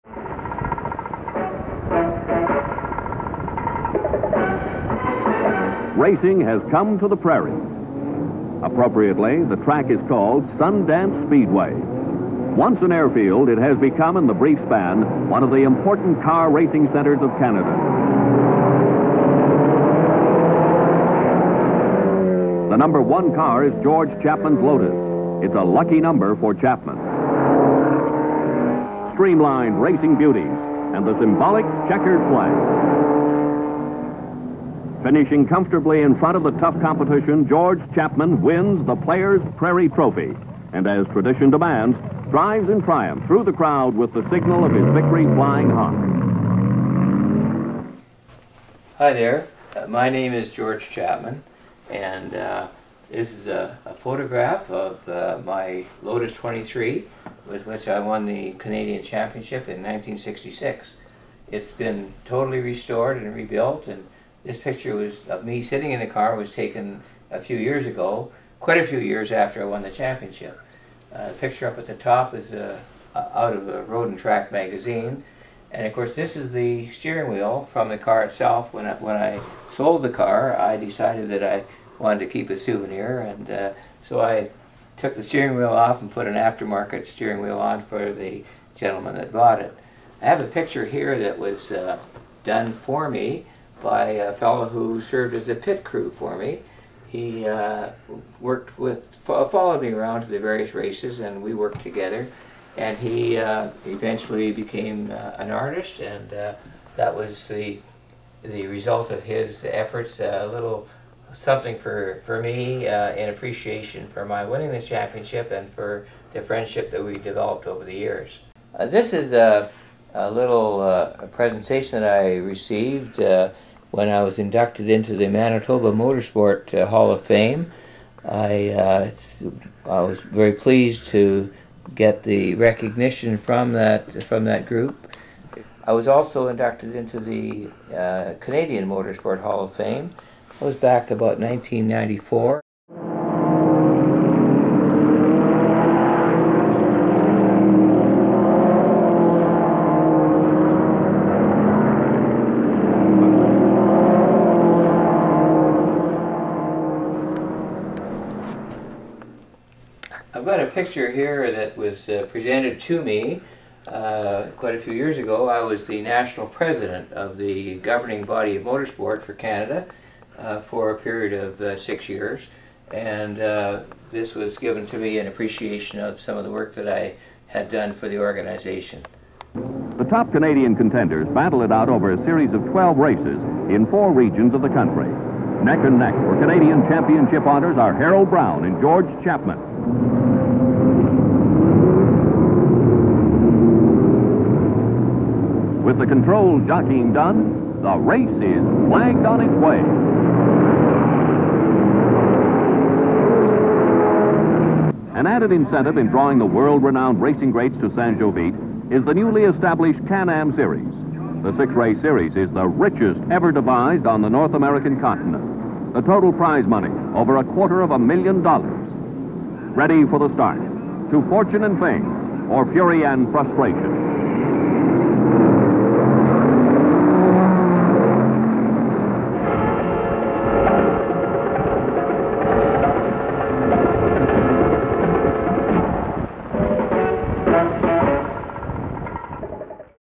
George Chapman
1966 Canadian Driving Champion, George Chapman, in Winnipeg, March 2011